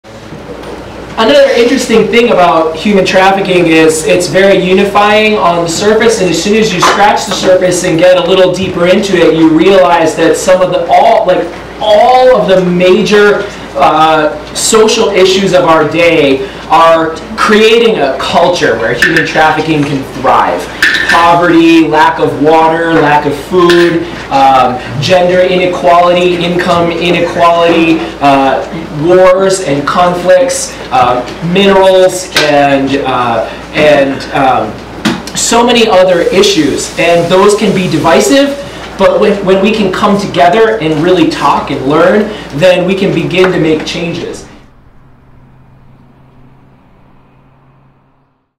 Shifting the Culture Surrounding Human Trafficking
Bryan Bessette, the director of The Freedom Café, talks about the importance of coming together to tackle the social issues that allow human trafficking to occur.